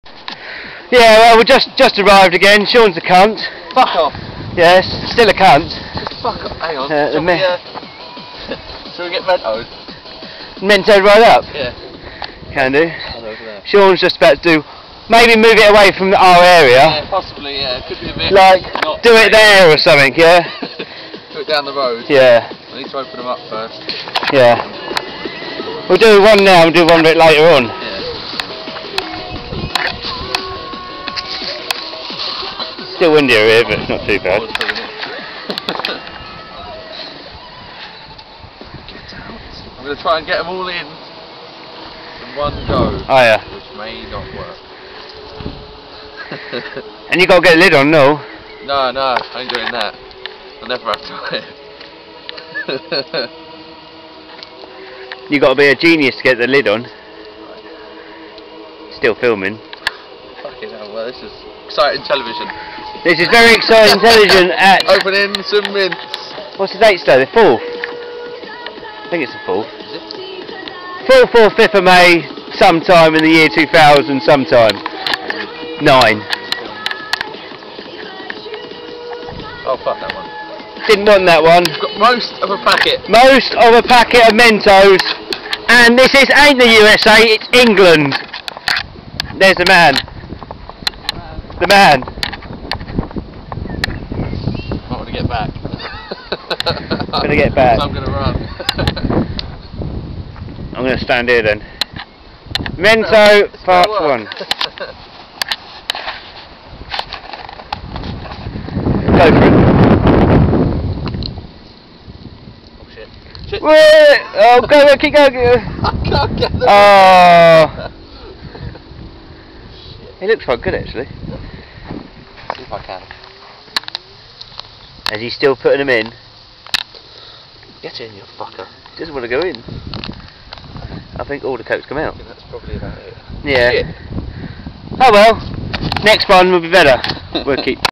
Diet Coke and Mentos Part 1
Sean was first to give it a try.